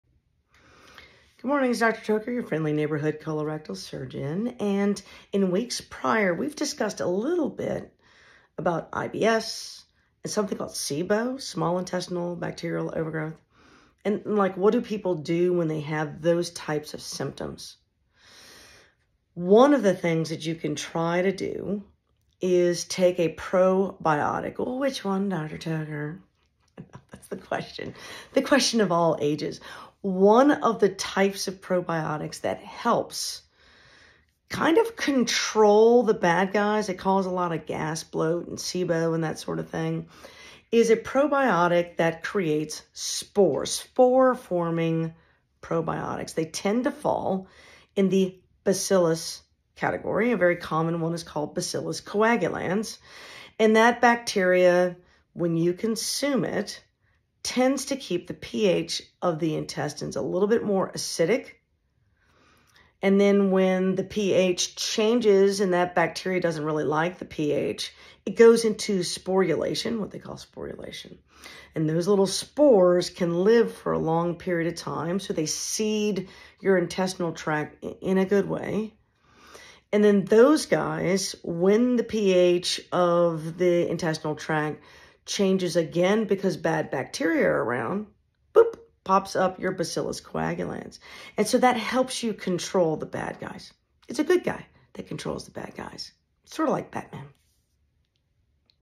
Spore Forming Probiotics
If you have abdominal bloat, cramping and bizarre bowel habits, it may be because of bacterial imbalances in the microbiome and a spore forming probiotic might be the answer you are searching for...